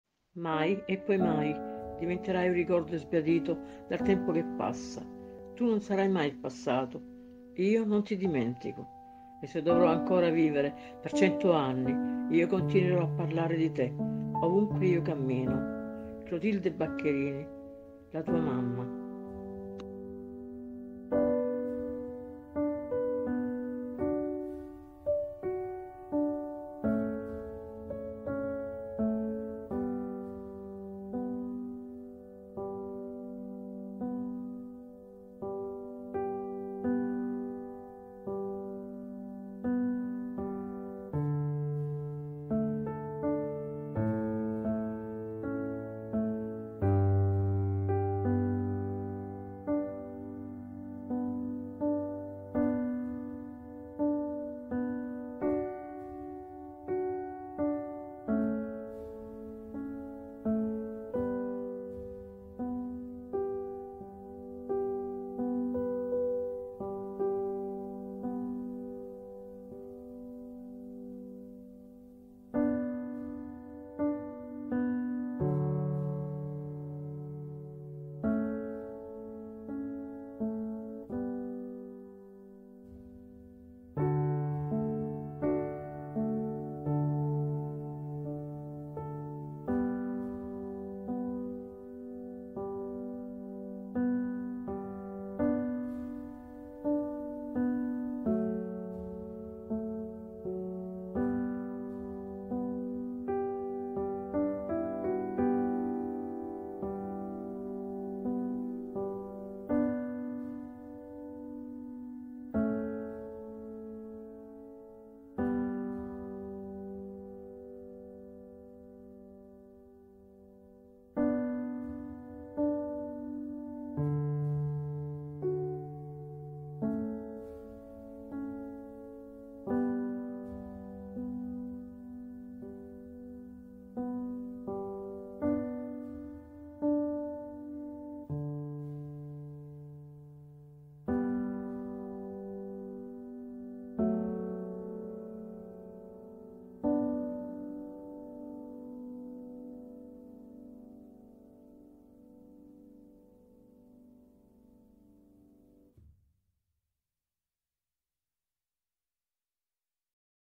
Clotilde Baccherini ♥️Poesie♥️
Mai e poi mai
    diventerai
un ricordo sbiadito
dal tempo che passa.

Tu non sarai mai
     il passato
Io non ti dimentico
e se dovrò ancora
vivere per cento
 anni io continuerò
    a parlare di te
ovunque io cammino.

Clotilde Baccherini
Poesia musica di Parole
Creative video
annare53 💕